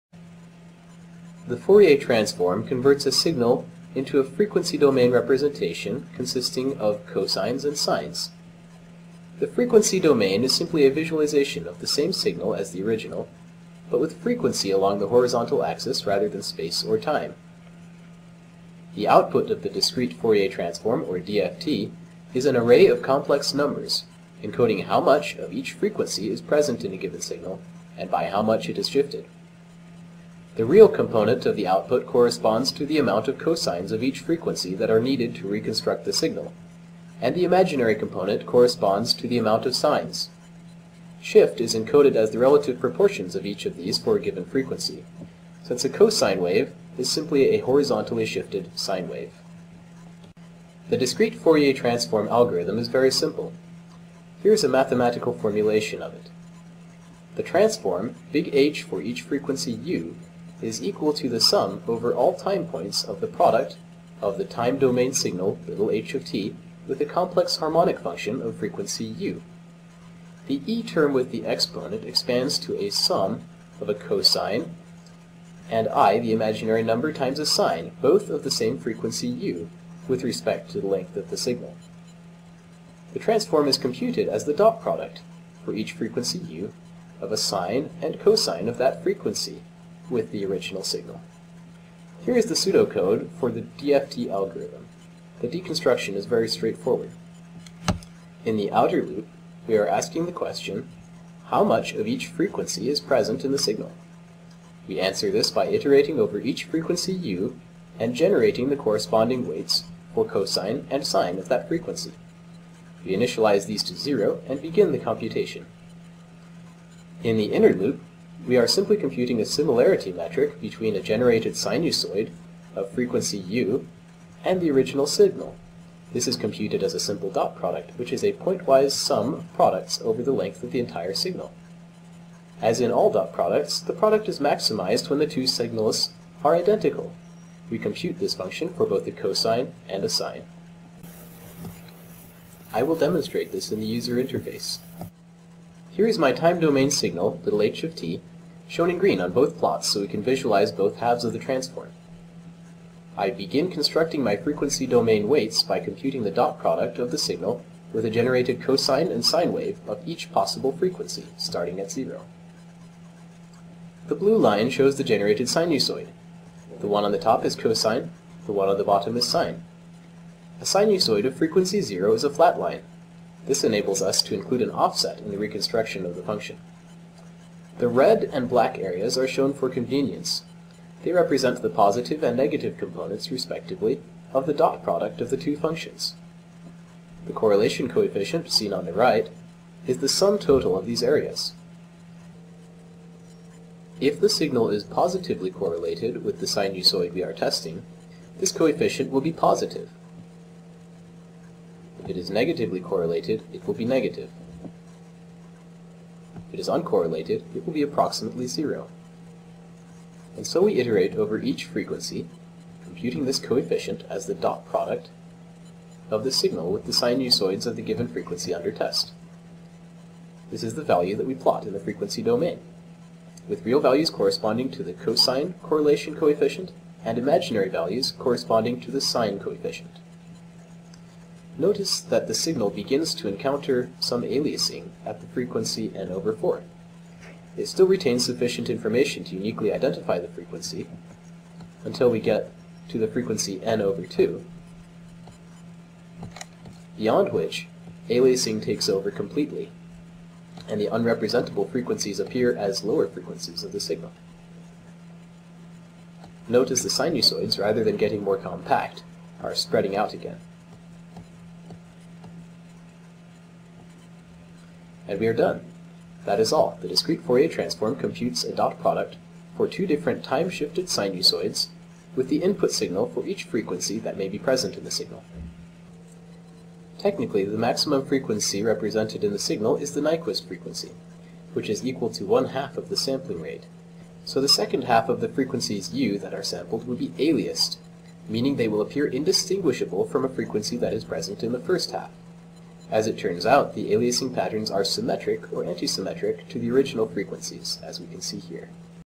Discrete Fourier Transform (DFT) Explained in 5 min
5-minute walkthrough explaining the Discrete Fourier Transform, going from the math, to the pseudocode, to the intuitive result. The outer loop of the code is stepped through, one frequency at a time, to see how and why it works.